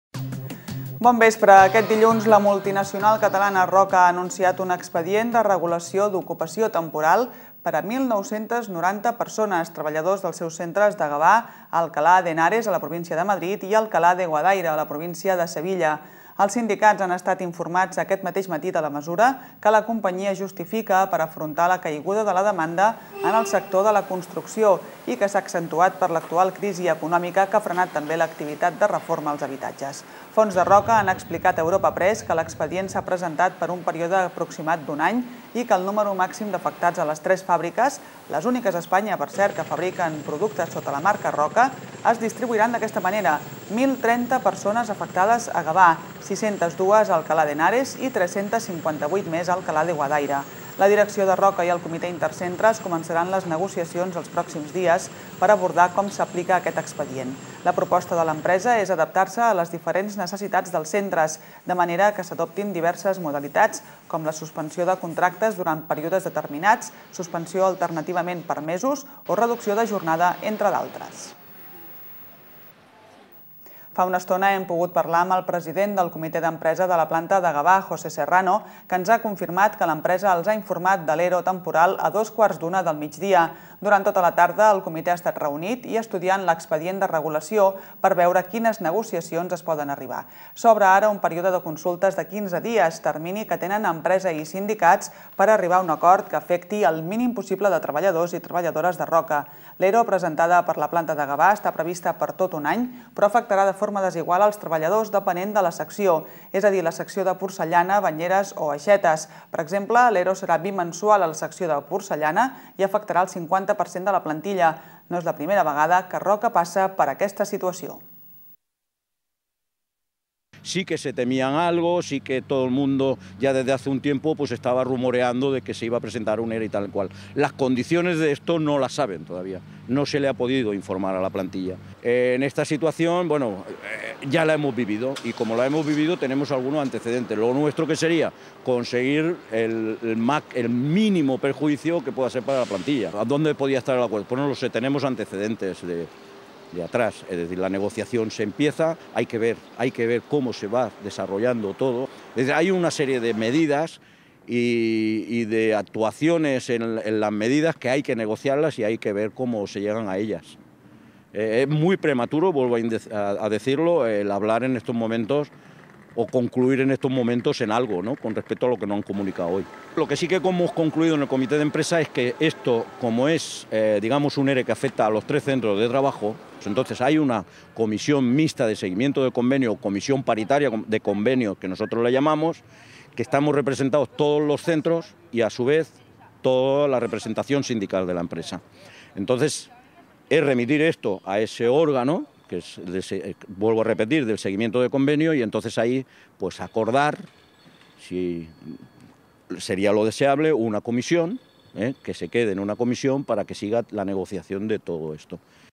GTV Notícies 19/01/09 - Roca presenta una ERO
Aquest dilluns la multinacional catalana Roca ha anunciat un Expedient de Regulació d'Ocupació (ERO) temporal per a 1.990 treballadors en els seus centres de Gavà, Alcalá de Henares (Madrid) i Alcalá de Guadaira (Sevilla).